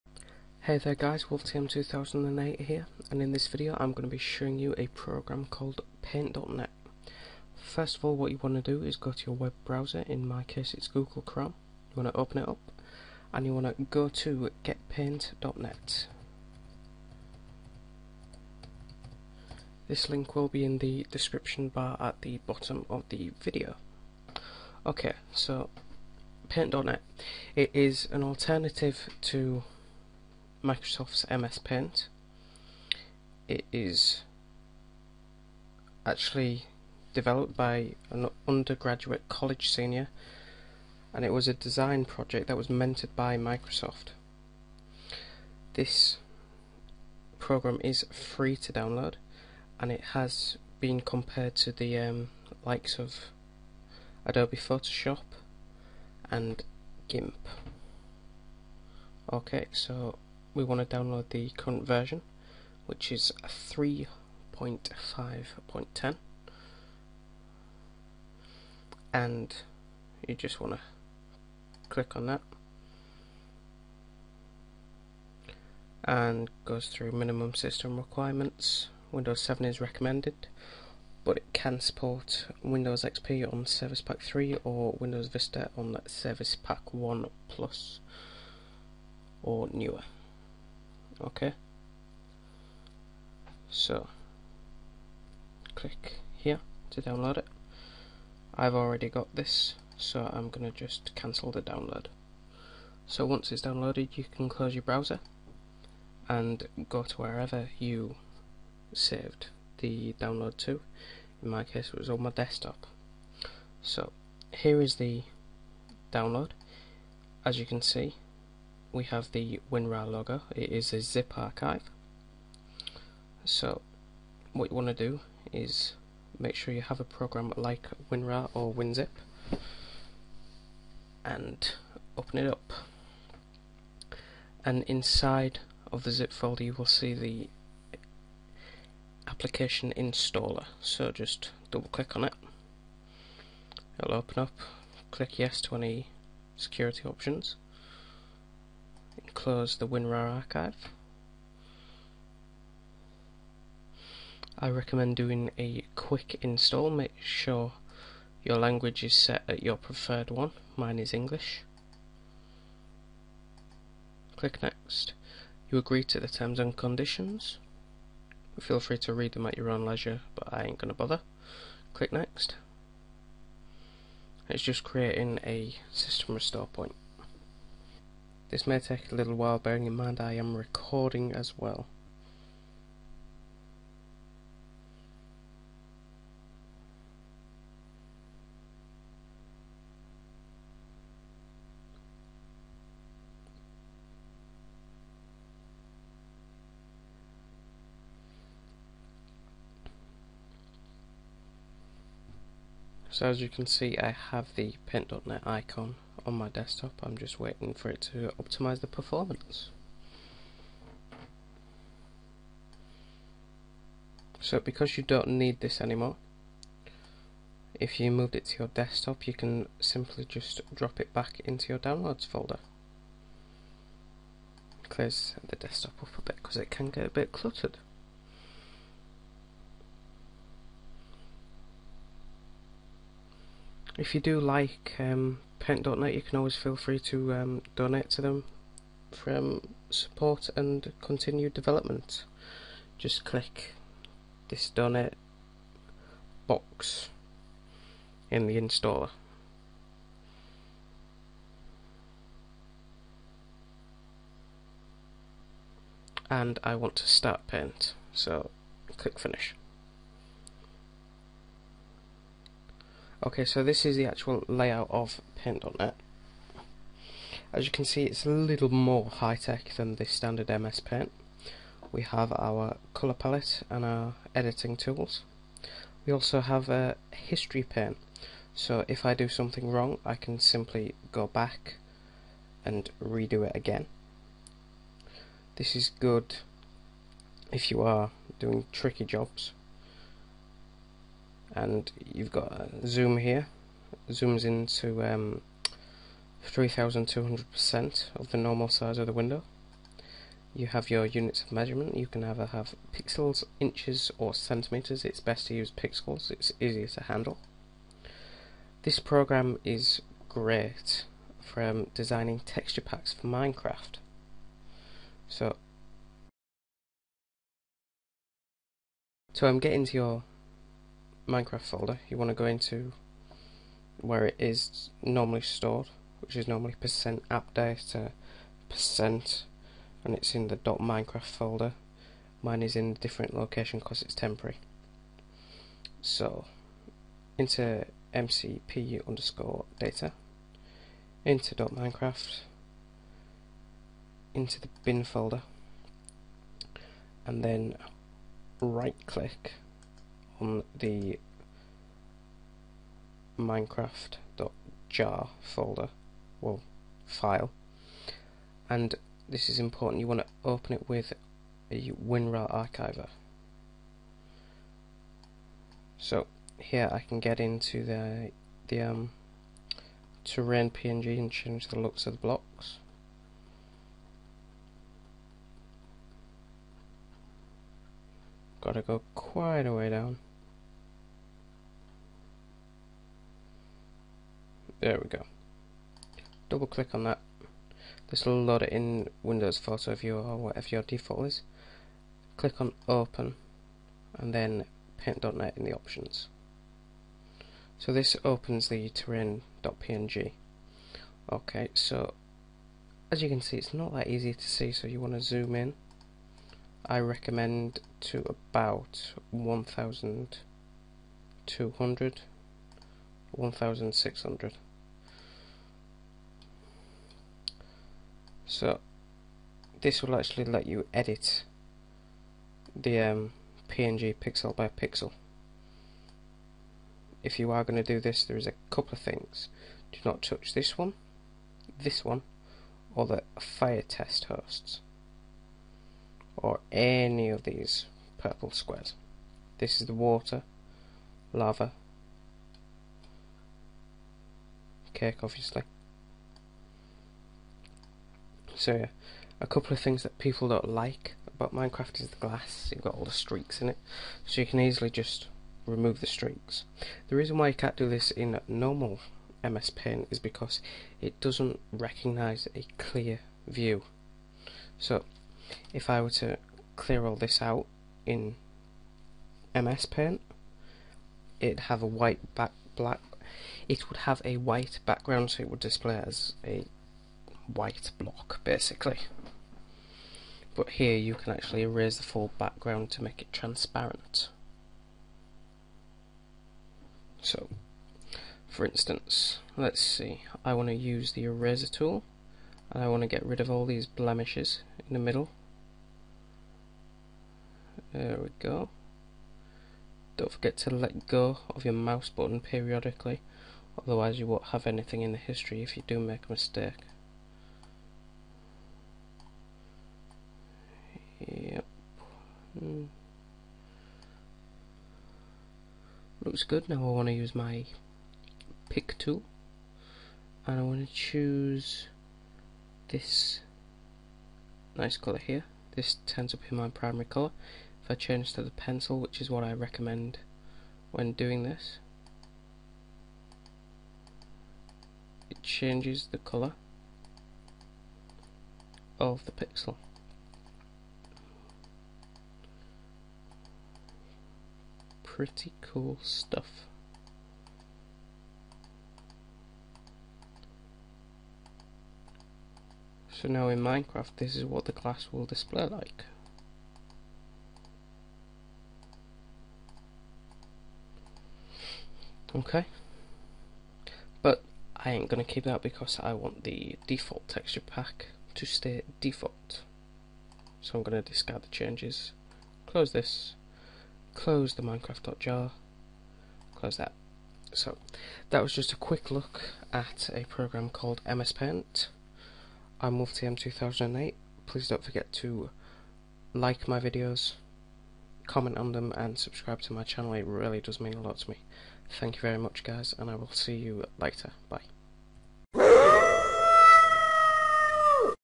Paint.net Vs MS Paint
PLEASE READ!!!
Hi Guys, in this video I will be talking a free program called Paint.net and why it is different to MS Paint